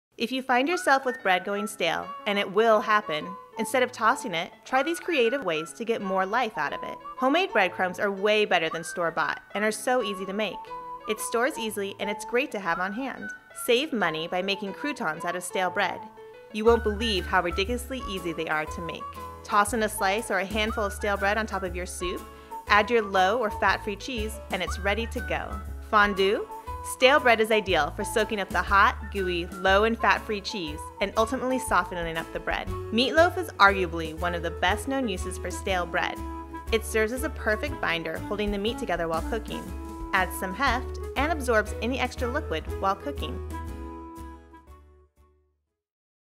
7 Ways to Use Bread
Don't throw it out! Here are 7 ways you can use stale bread.